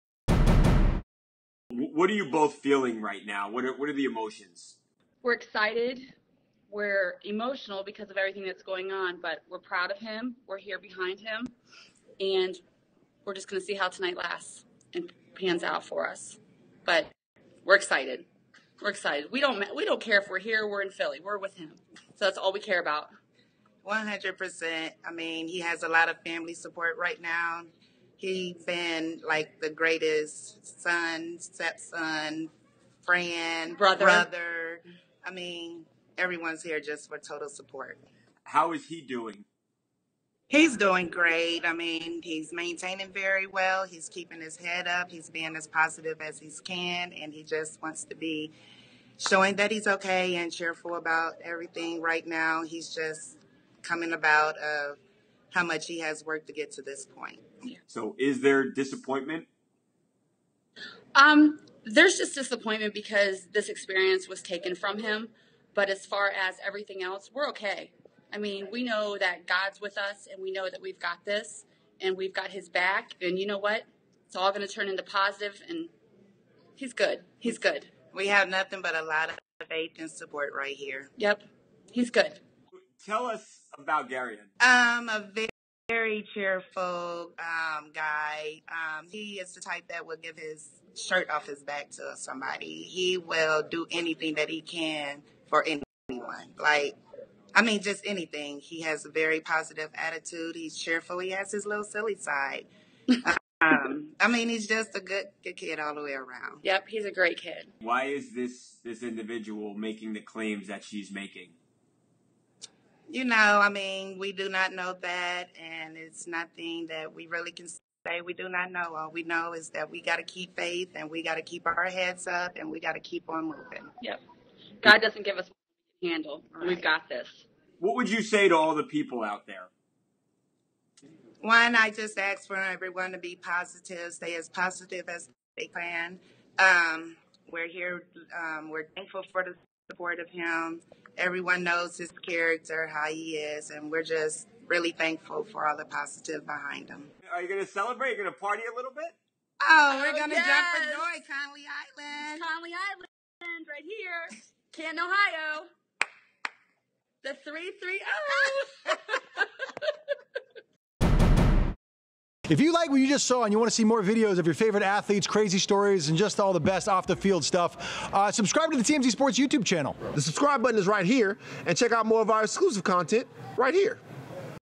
Gareon Conley At Family Draft Party, 'God's With Us' | TMZ Sports
Gareon Conley is not in Philadelphia at the NFL Draft -- instead he is with close friends and family at a private gathering in Ohio ... and he's doing "great" ... so says his mother and step-mother..